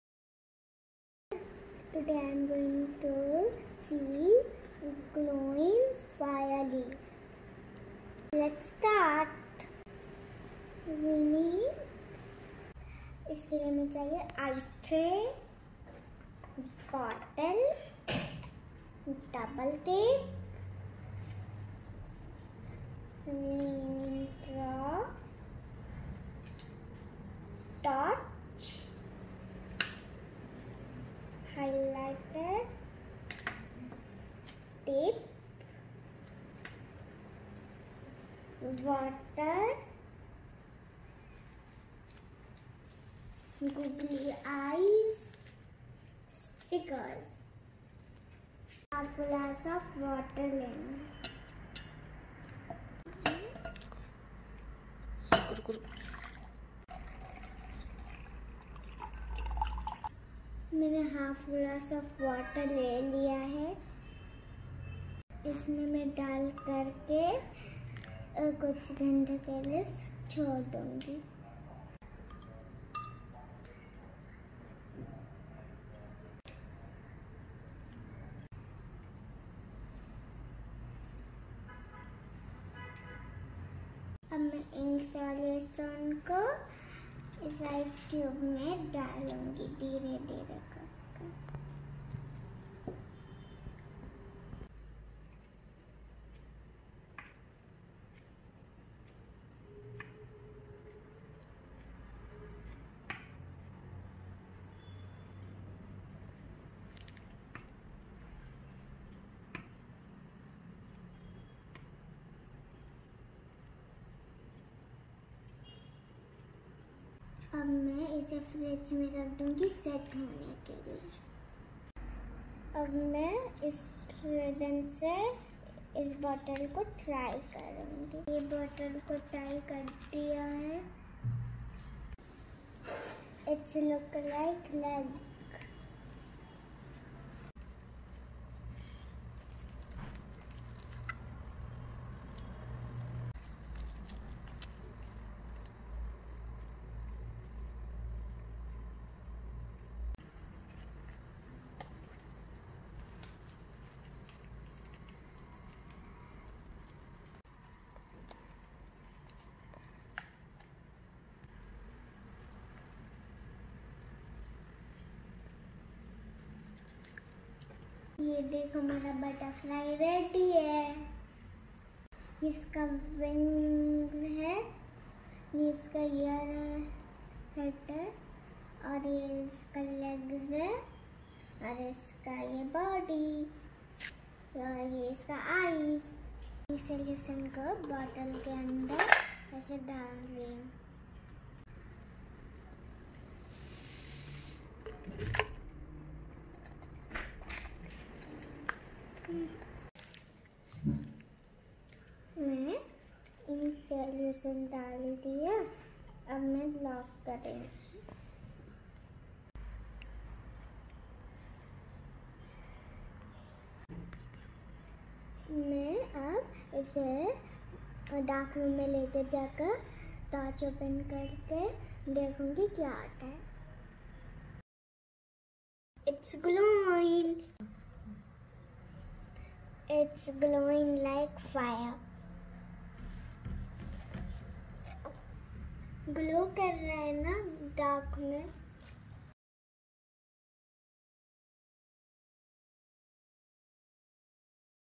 Glowing Firefly | Fun with Liquids | Hands-on Exploration with the science of Liquids
The ice cubes glowed in a dark room, because the highlighter had fluorescent ink. Fluorescent  objects absorbs or take- in light and re-emit a much stronger light.

Astounding Sound Experiment